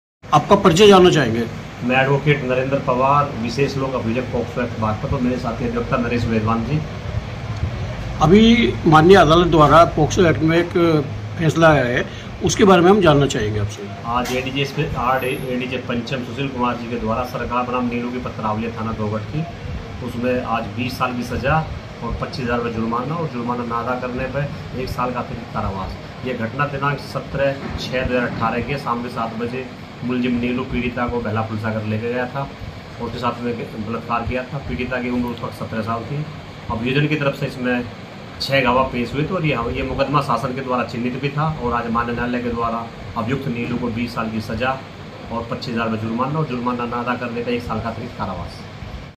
बागपत मे आरोपी को 20 वर्ष की सजा तथा 25000 रु० का जुर्माना*
*बागपत ब्रेकिंग!*
*जनपद बागपत मे नाबालिग के साथ दुष्कर्म के आरोपी को 20 वर्ष की सजा तथा 25000 रु० का जुर्माना*
*“ऑपरेशन कन्विक्शन” अभियान के क्रम में नाबालिग लडकी को बहला-फुसलाकर ले जाने व दुष्कर्म करने वाले चिन्हित अभियोग के आरोपी नीलू को पुलिस और अभियोजन विभाग द्वारा प्रभावी पैरवी किए जाने पर हुआ 20 वर्ष का कठोर कारावास व 25 हजार रूपये अर्थदण्ड की सजा ।*
     बाईट नरेन्द्र पवांर सरकारी वकील!
बागपत से सुनिल अग्रवाल!